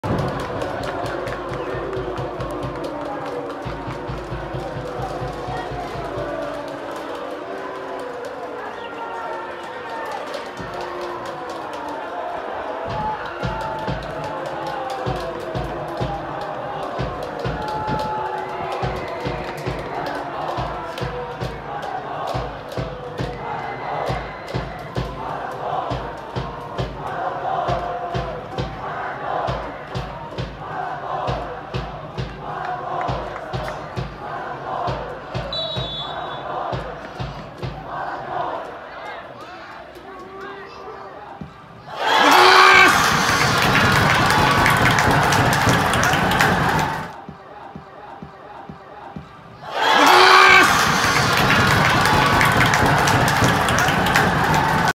千葉明徳VS日体大柏
千葉明徳のPKシーン。